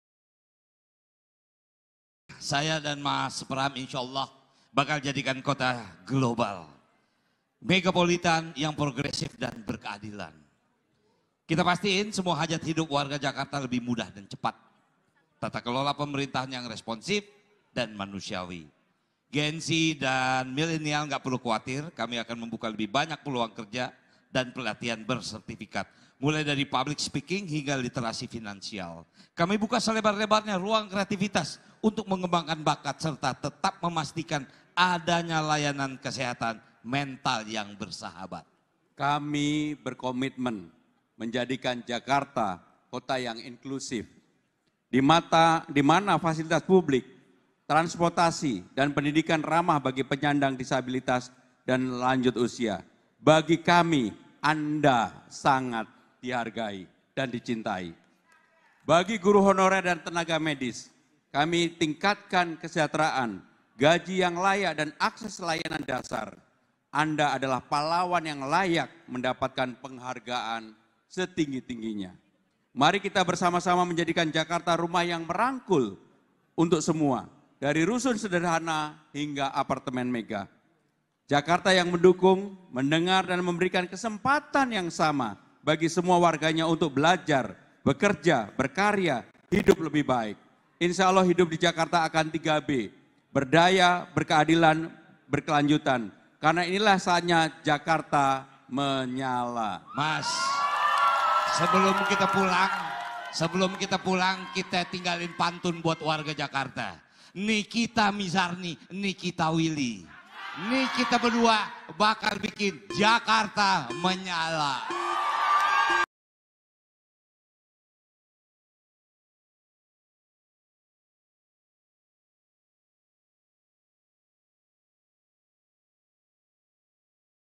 TUTUP DEBAT, PRAMONO-RANO INGIN BUAT JAKARTA MENYALA!
Menutup gelaran debat perdana Pilkada Jakarta, Pasangan Cagub dan Cawagub nomor urut 3, Pramono Anung dan Rano Karno menyampaikan kata penutupnya. Rano Karno menyampaikan agar para Gen Z dan Milenial tidak perlu khawatir soal peluang pekerjaan. Pramono Anung berkomitmen agar Jakarta menjadi kota yang inklusif. Pramono berharap agar Jakarta menjadi kota yang merangkul bagi semua warganya.